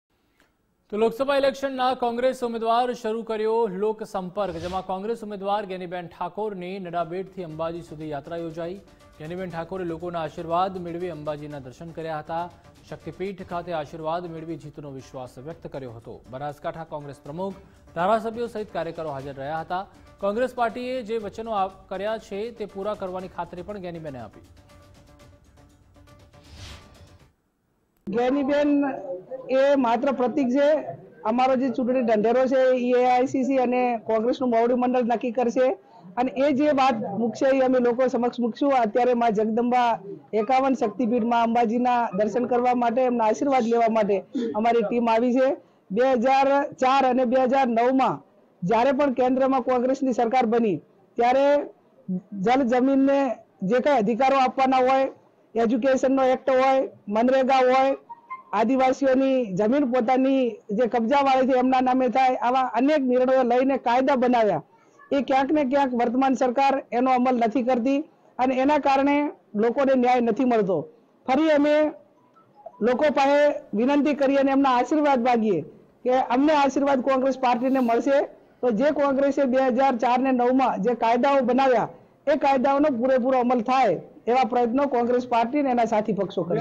Banaskantha: કોંગ્રેસ ઉમેદવાર Geniben Thakorની નડાબેટથી અંબાજી સુધી યાત્રા
લોકસભા ચૂંટણીના કોંગ્રેસ ઉમેદવાર શરૂ કર્યો લોક સંપર્ક
કોંગ્રેસ ઉમેદવાર ગેનીબેન ઠાકોરની નડાબેટથી અંબાજી સુધી યાત્રા
ગેનીબેન ઠાકોરે લોકોના આર્શિવાદ મેળવી અંબાજીના દર્શન કર્યા